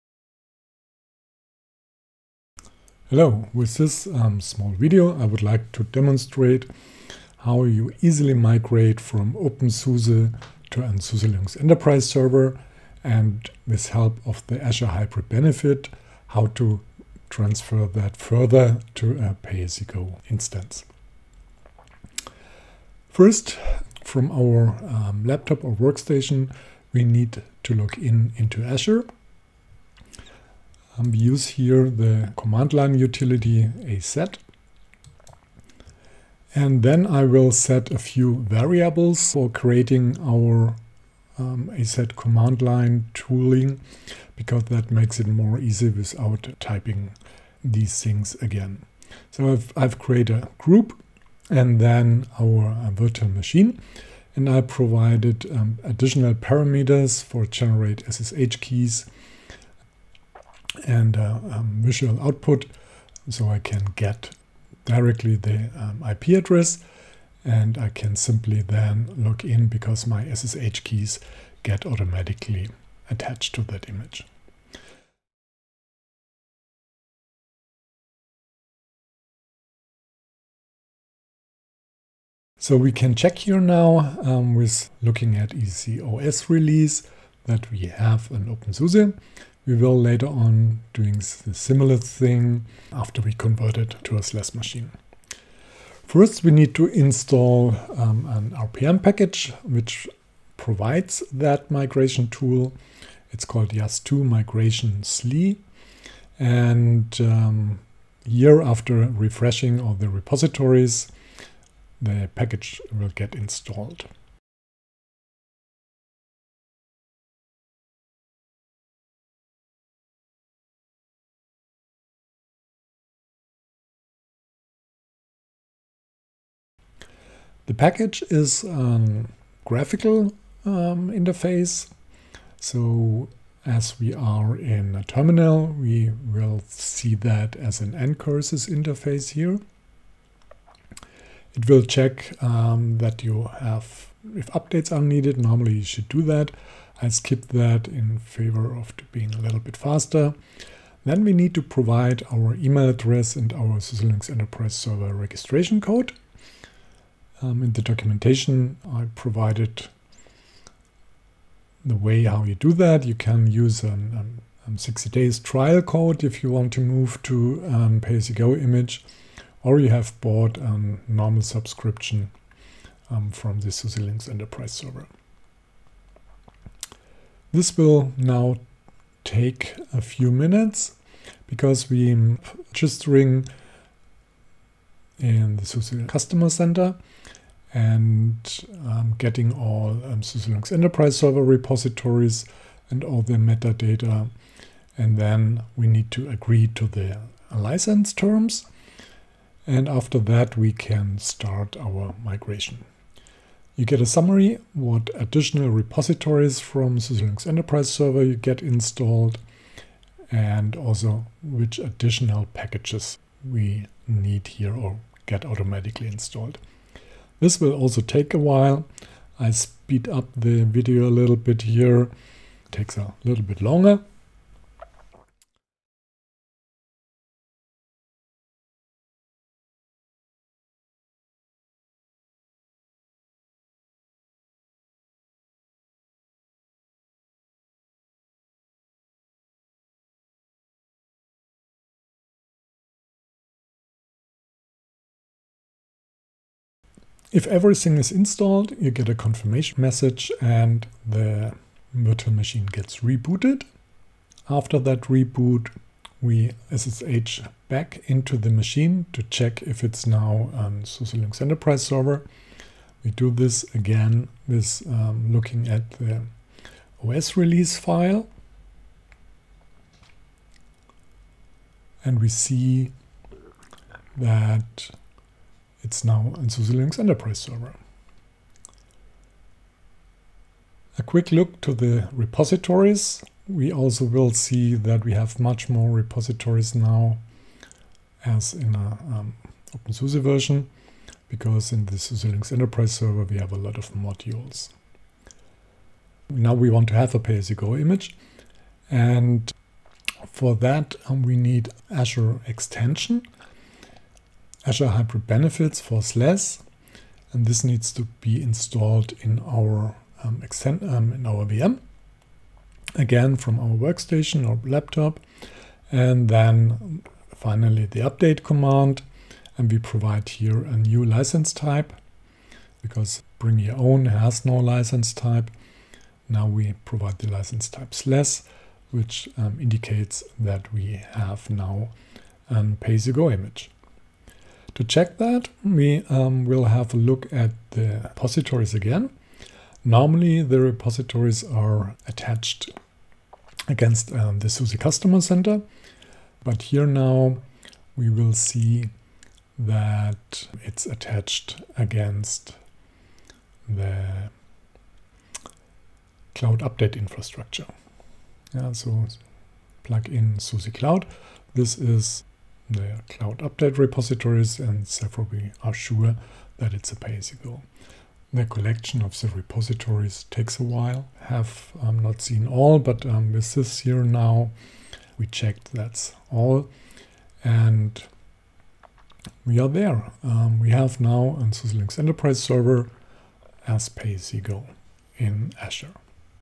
openSUSE to SUSE Linux Enterprise Migration
This video showcases the migration of openSUSE Leap to SUSE Linux Enterprise Server, followed by the conversion of the running system from BYOS to PAYG in Azure.